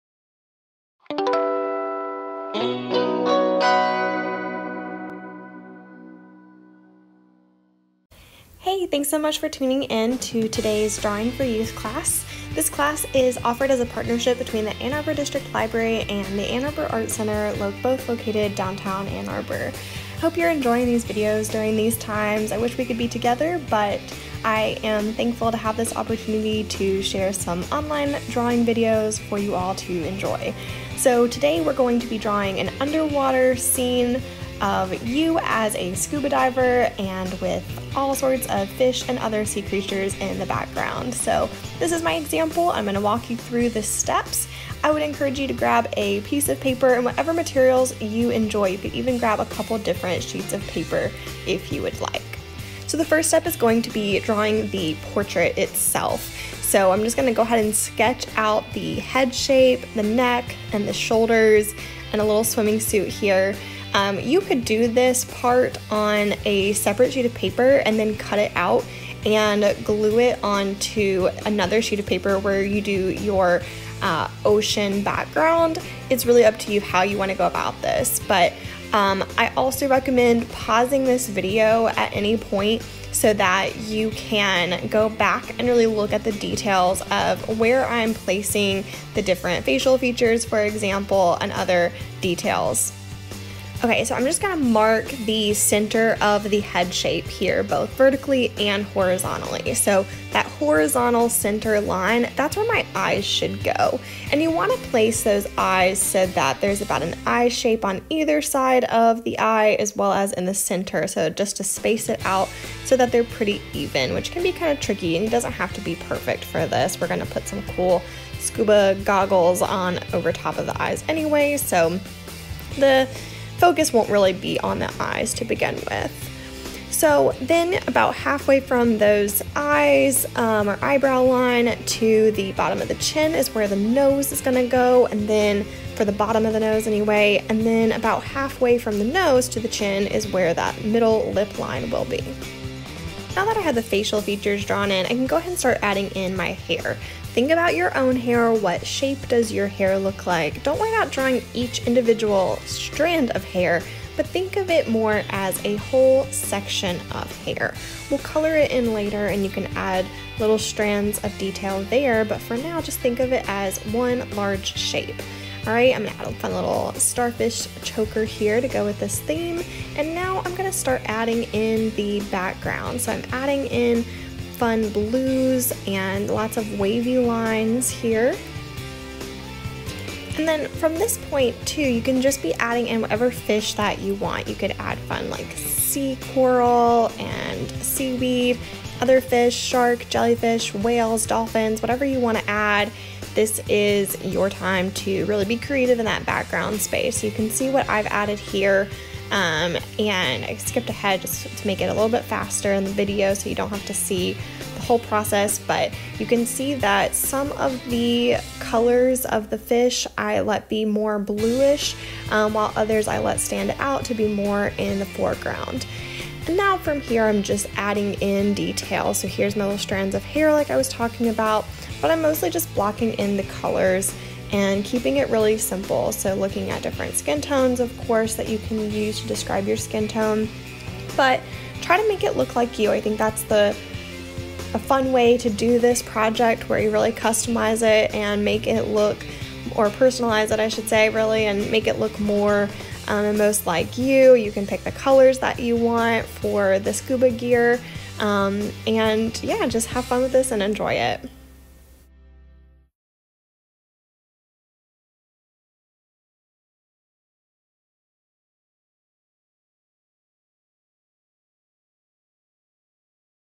Drawing for Kids | Underwater Adventure Drawing
SUMMERGAME2020: THATSAMORAY With all of this hot summer weather, you must be ready to cool off and go for a swim! Join Payton and design your own underwater self-portrait. What fantastic sea creatures will be swimming past you in your artwork? For this activity, you will need a pencil, paper, eraser, and either colored pencils, crayons, or markers.  As always, if you don’t have everything on this list, don’t worry – feel free to be creative and find substitutions for any supplies you may not have on hand.